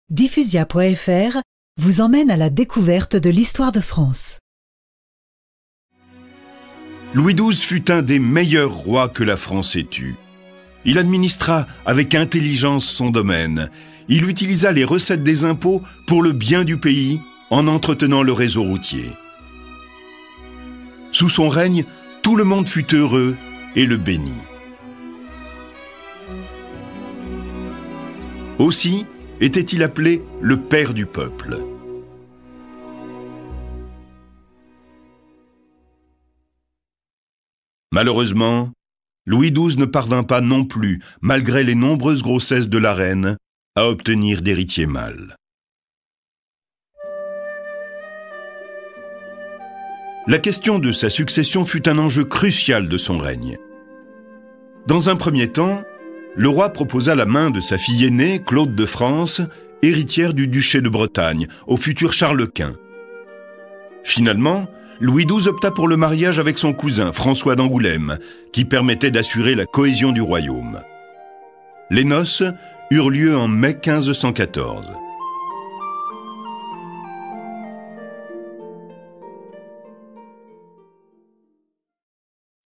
Le 18 mai 1514, François Ier épouse Claude de France, fille du roi de France Louis XII
Cet épisode de l'histoire de France, raconté par Marc Brunet, est présenté dans le CD François Ier le Roi de la Renaissance disponible sur Diffusia.fr